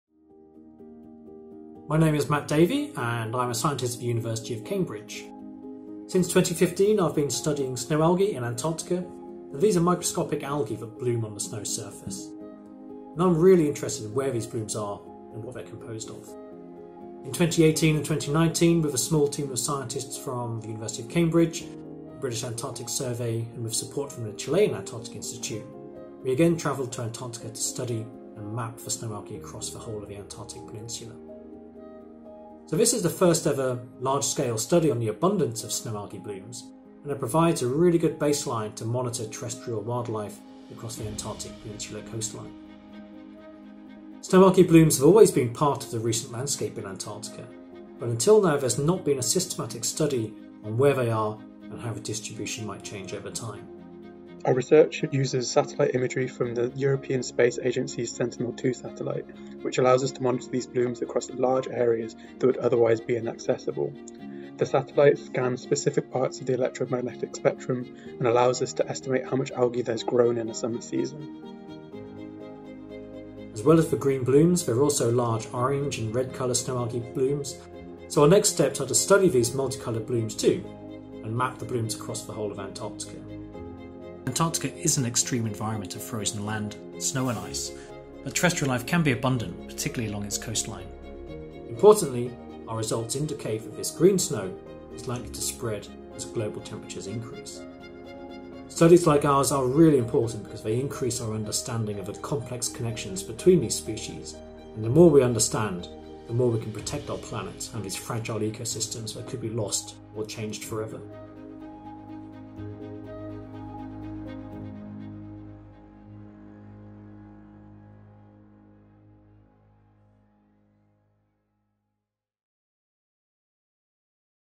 Green Antarctica
Scientists have created the first ever large-scale map of microscopic algae as they bloomed across the surface of snow along the Antarctic Peninsula coast. Results indicate that this ‘green snow’ is likely to spread as global temperatures increase.